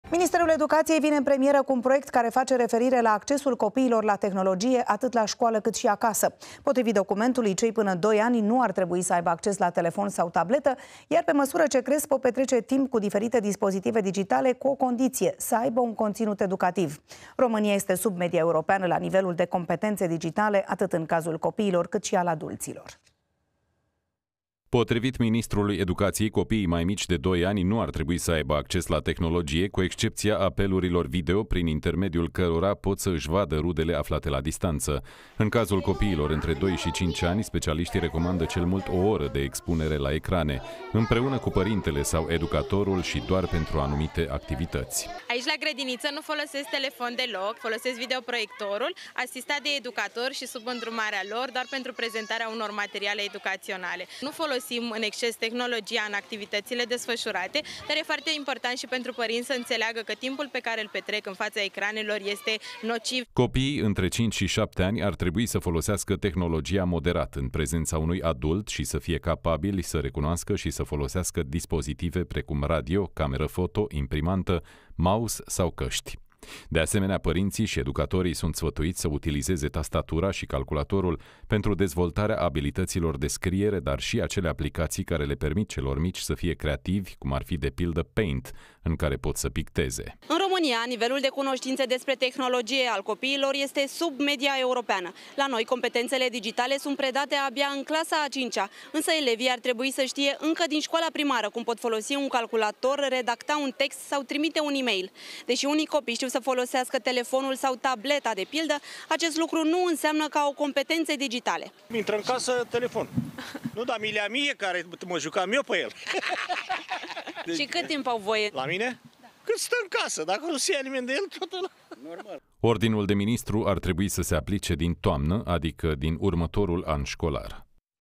Lege de la Ministerul Educației privind accesul copiilor la tehnologie
Ministerul Educației vine în premieră cu un proiect care face referire la accesul copiilor la tehnologie, atât la școală, cât și acasă.